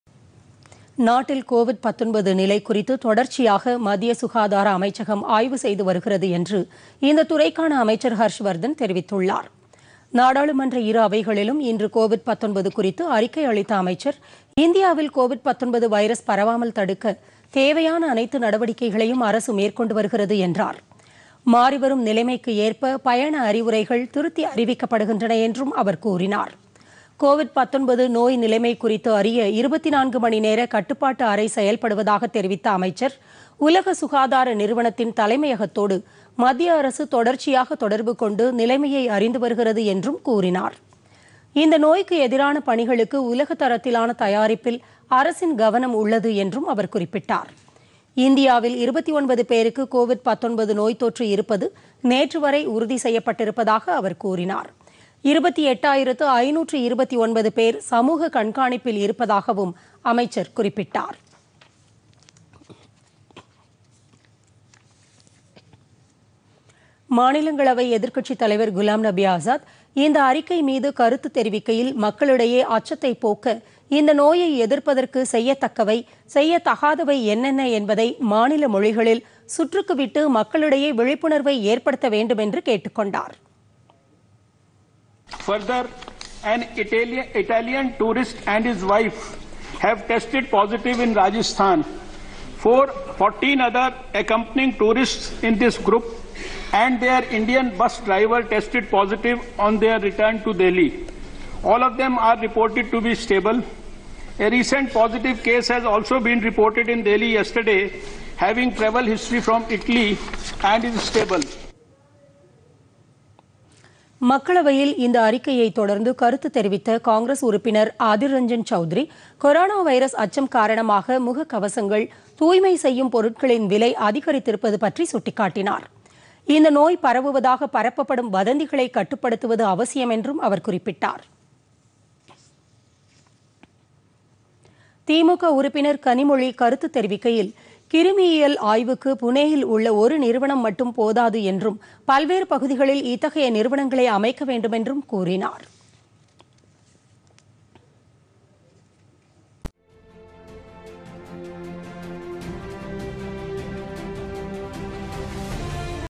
கோவிட் 19 - நாடாளுமன்றத்தில் அறிக்கைPodhigaiTamilNews
கோவிட் 19 வைரஸ் பரவாமல் தடுக்க தேவையான அனைத்து நடவடிக்கைகளும் மேற்கொள்ளப்படுகின்றன - நாடாளுமன்றத்தில் சுகாதார அமைச்சர் ஹர்ஷ்வர்தன் அறிக்கை.